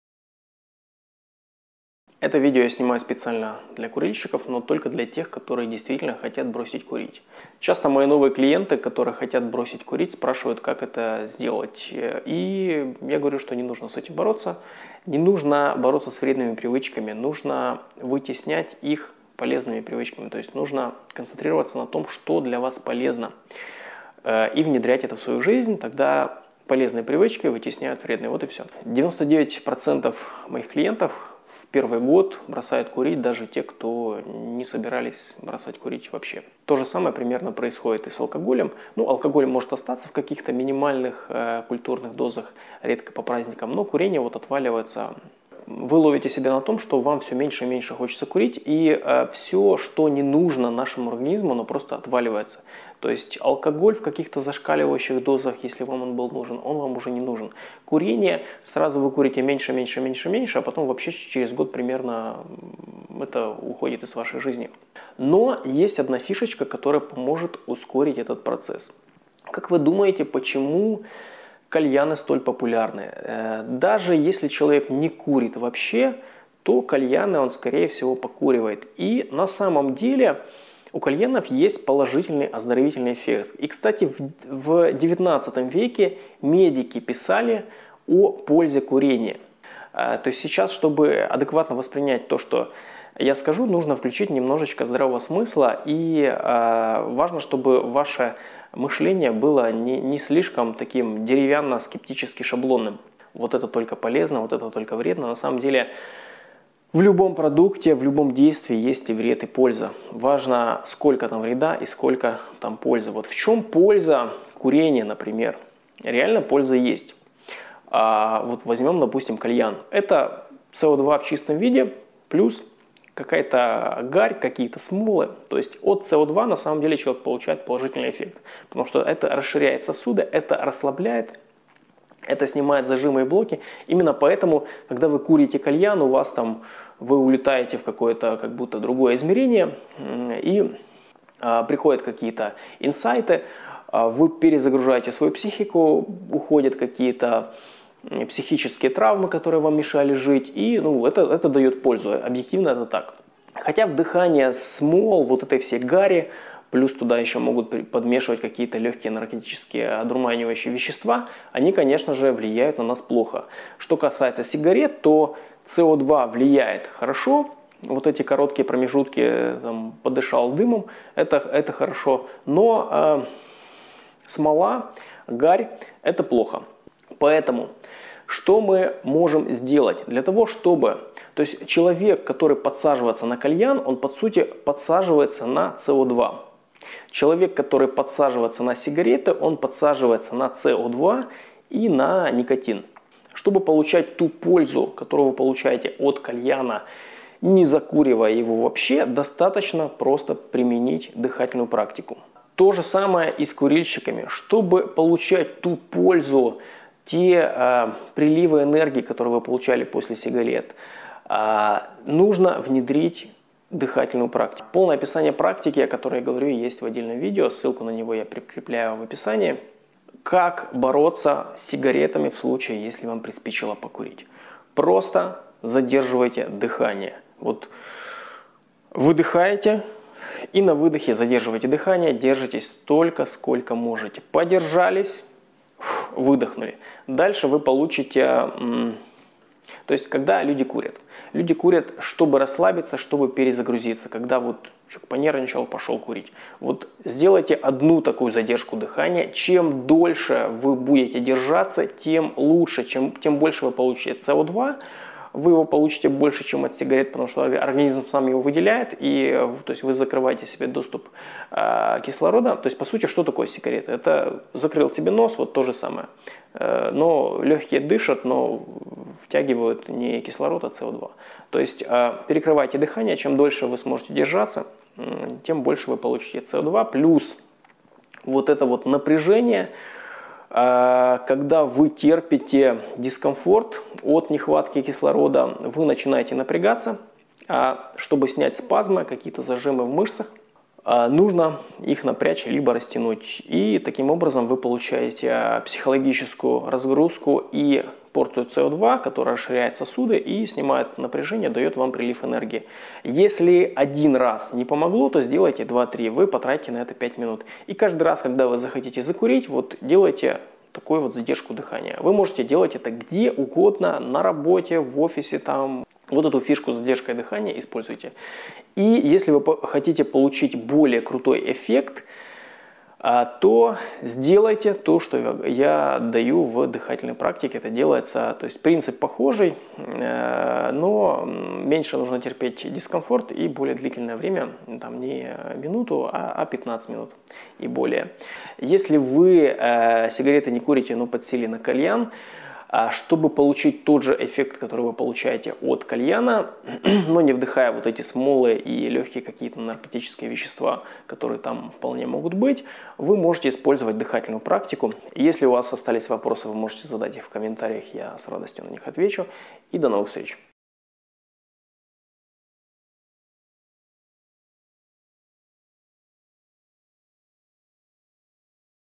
Как бросить курить - рабочий метод
01:52 О пользе и вреде кальяна
03:12 Чем заменить кальян и сигареты
03:40 Как бороться с сигаретами, если вам приспичило покурить